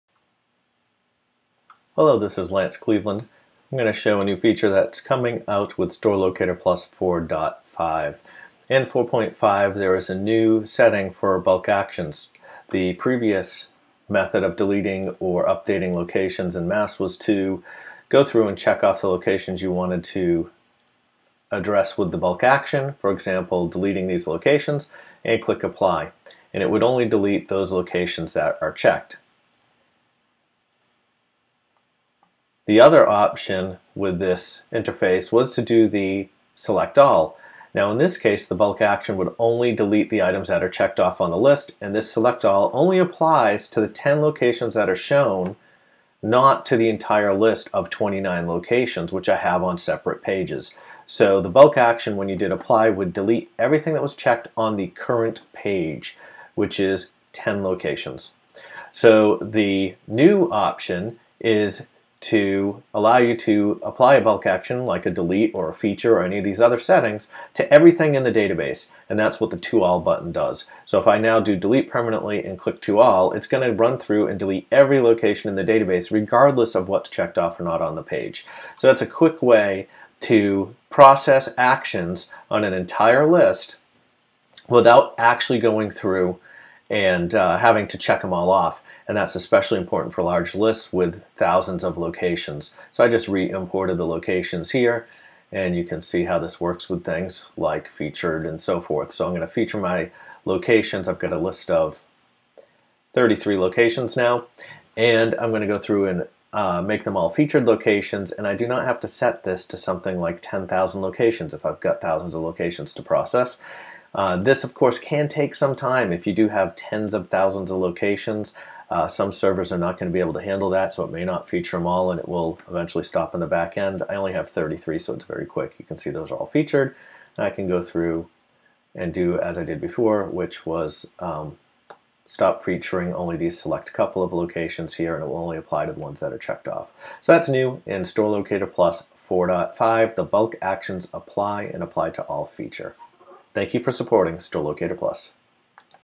Store Locator Plus 4.5 Bulk Actions Apply To All
SLP 4.5 introduces a new "Apply To All" button on the manage locations interface.  You no longer need to list all 10,000 locations and check them off to to mass updates.  You can now select the action and pick the "to all" part of the apply to all button.